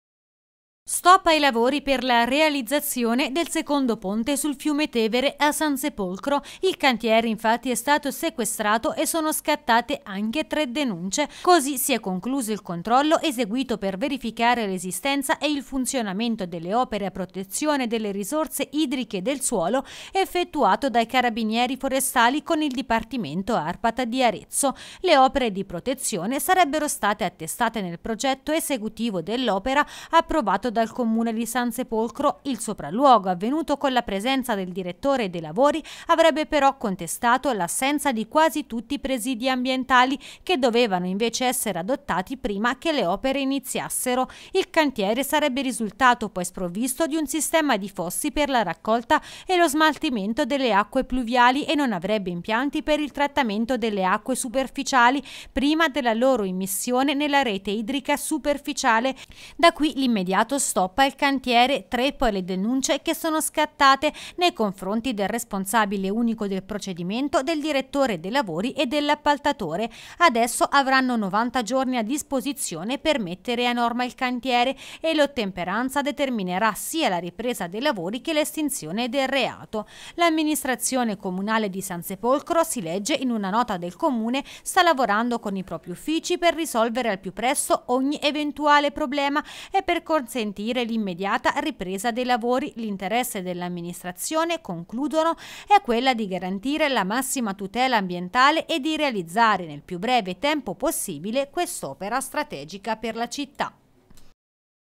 Sansepolcro, stop ai lavori per il secondo ponte sul Tevere: Forestali denunciano tre persone
Il cantiere per la realizzazione del secondo ponte sul Fiume Tevere, a Sansepolcro, è stato sequestrato provocando oltre alla sospensione dei lavori, tre denunce. Il controllo, eseguito per verificare l'esistenza e il funzionamento delle opere a protezione "delle risorse idriche e del suolo" è stato effettuato dai carabinieri forestali con il dipartimento Arpat di Arezzo. Le opere di protezione sarebbero state attestate nel progetto esecutivo dell'opera, approvato dal Comune di Sansepolcro. Il sopralluogo, avvenuto con la presenza del direttore dei lavori, avrebbe constatato l'assenza di quasi tutti i presidi ambientali che dovevano essere adottati prima che le opere iniziassero. Il cantiere sarebbe risultato sprovvisto di un sistema di fossi per la raccolta e lo smaltimento delle acque pluviali e non avrebbe impianti per il trattamento delle acque superficiali, prima della loro immissione nella rete idrica superficiale. Inoltre, lo scarico dei reflui in acque superficiali, non risulterebbe autorizzato. Per questi motivi la polizia giudiziaria ha prescritto l'immediata sospensione dei lavori fino alla preventiva realizzazione delle opere indicate. Inoltre sono state emesse tre denunce, verso il responsabile unico del procedimento, e nei confronti del direttore dei lavori e l'appaltatore. Saranno 90 i giorni che avranno a disposizione per mettere a norma il cantiere e l'ottemperanza determinerà sia la ripresa dei lavori che l'estinzione del reato.